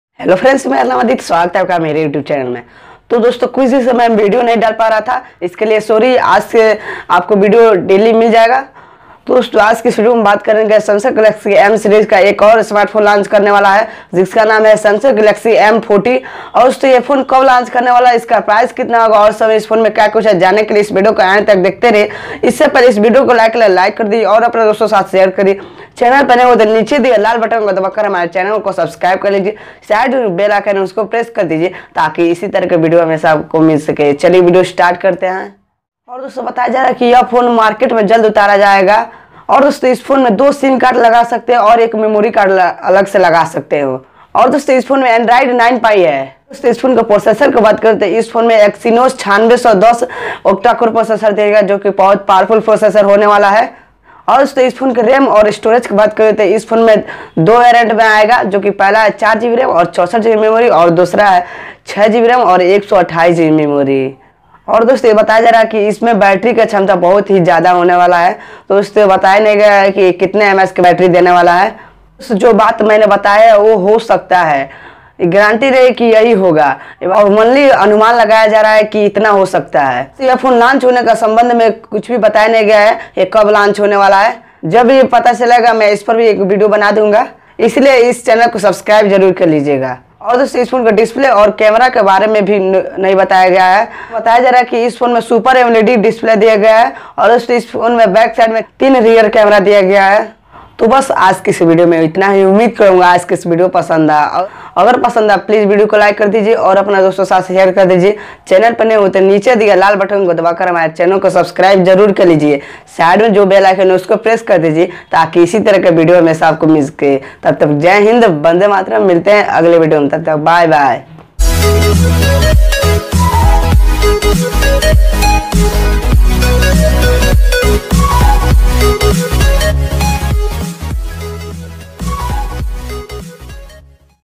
Samsung Galaxy M40- Launch Date, Price, Camera, Specifications In India
Samsung Galaxy M40-Launch Date, Price, Camera,Specifications In India| Samsung
Galaxy M40

My first Mic

Gorilapod Tripod For Vlog

Light Stand

Airtel 4G Hotspot